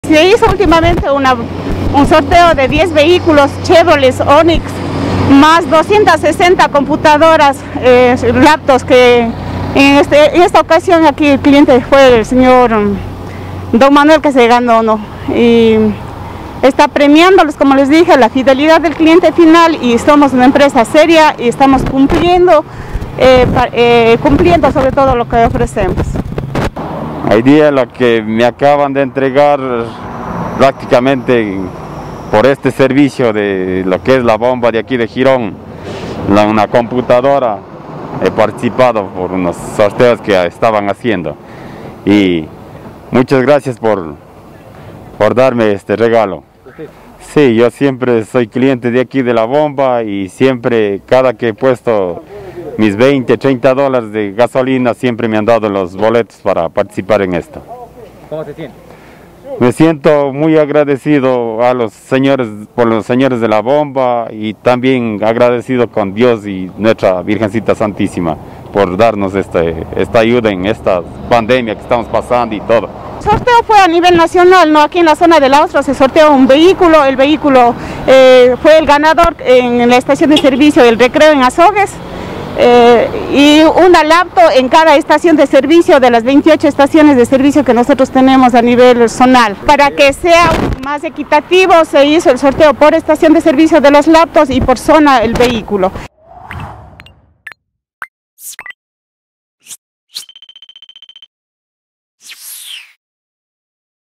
Manuel Tenecota ganó una computadora en el sorteo de la estación de servicios
Este jueves se realizó la entrega del premio ganado por el señor Manuel Tenecota, quien es cliente de la estación de servicio localizada en Girón. A finales del año 2020, en el lugar, por la carga de combustible se entregaban boletos para un sorteo de varios premios, a uno de ellos se hizo acreedor el gironense. Una computadora, un regalo muy apreciable en tiempos de pandemia resaltó el feliz ganador.